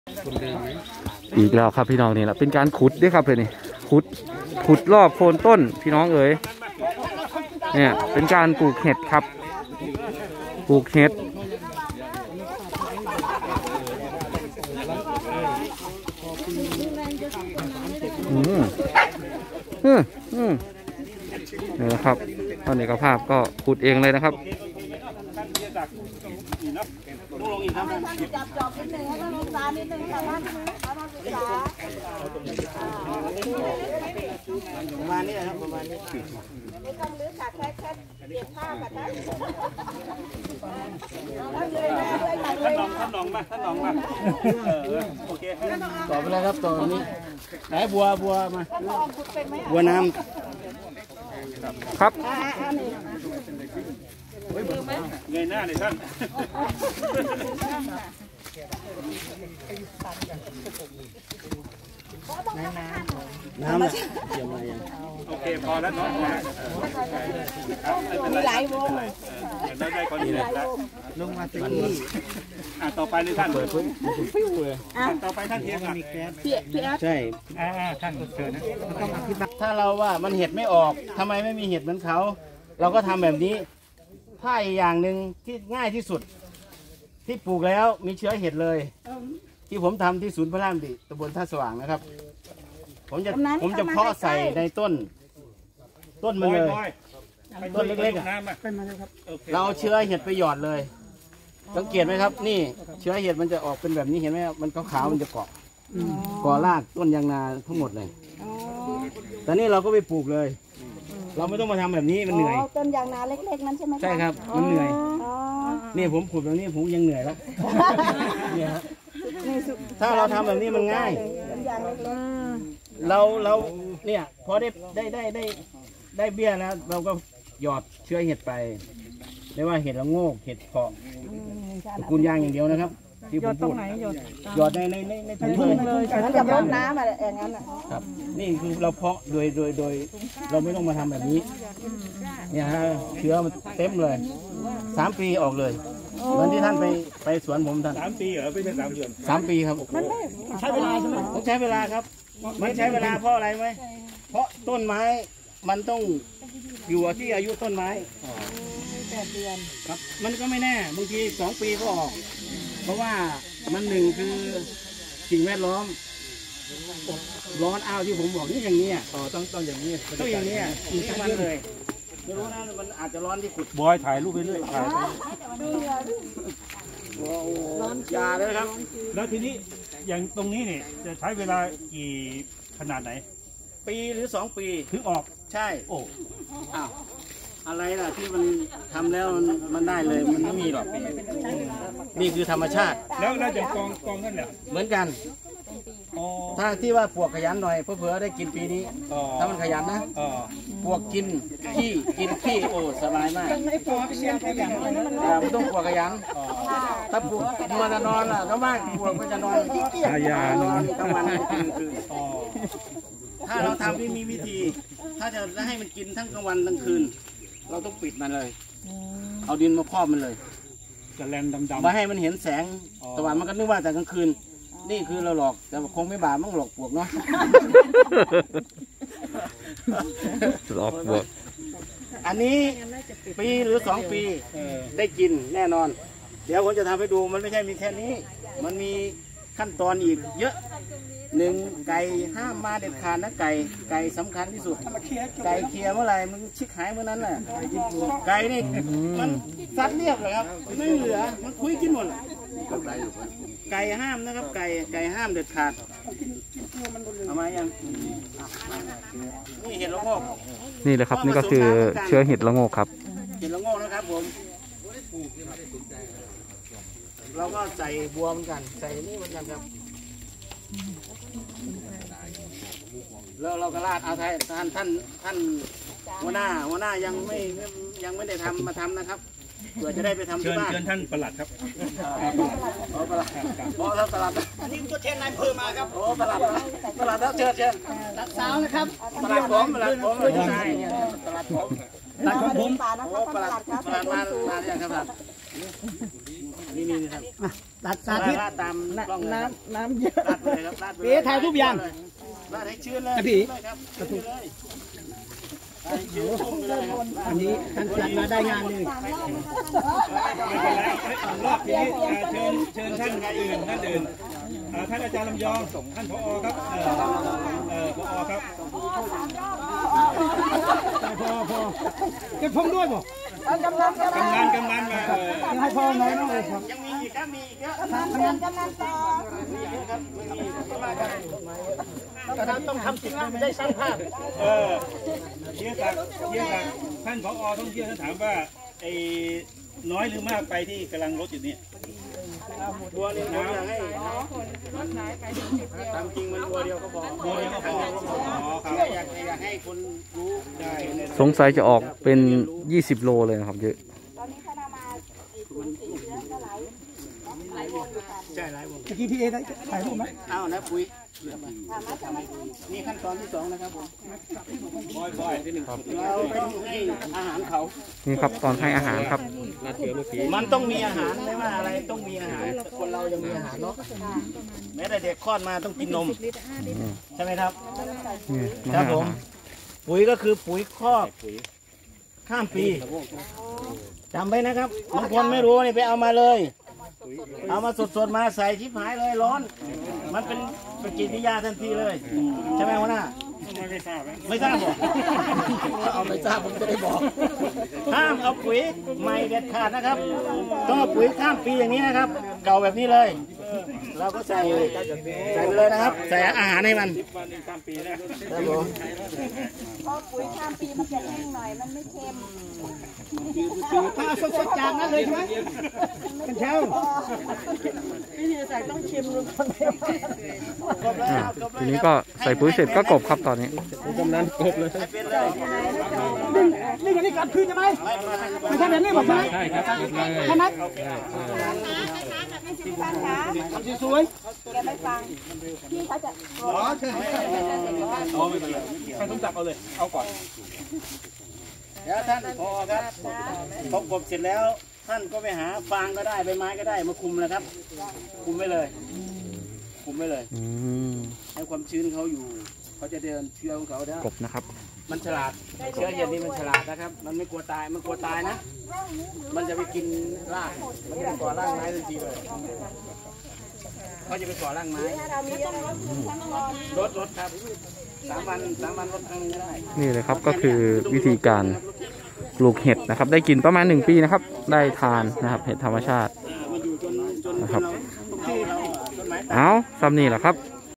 กอ.รมน.สุรินทร์ นำทีมพาชมวิธีเพราะเห็ด ที่ ร.ร.สุรินทร์ภักดี
วีธีการเพาะเชื้อเห็ด พึ่งพาธรรมชาติ